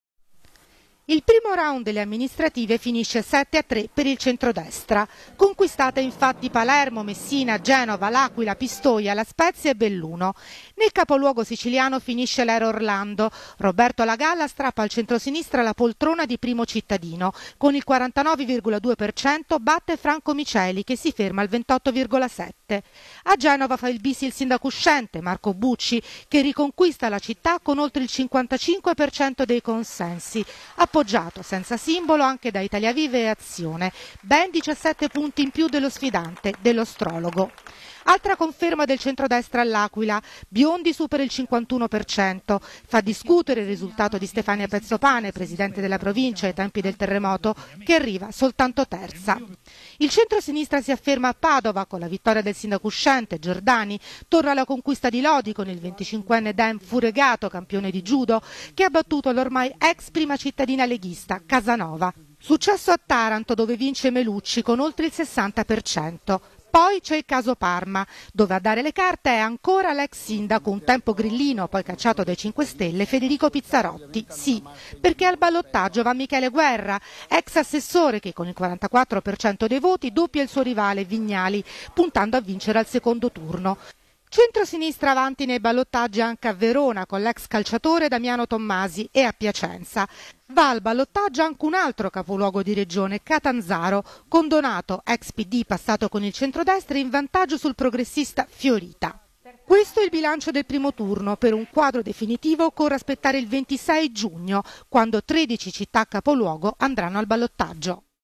Comunali, vincitori e vinti
Il primo round delle amministrative finisce 7 a 3 per il centrodestra. Conquistate infatti Palermo, Messina, Genova, L'Aquila, Pistoia, La Spezia e Belluno.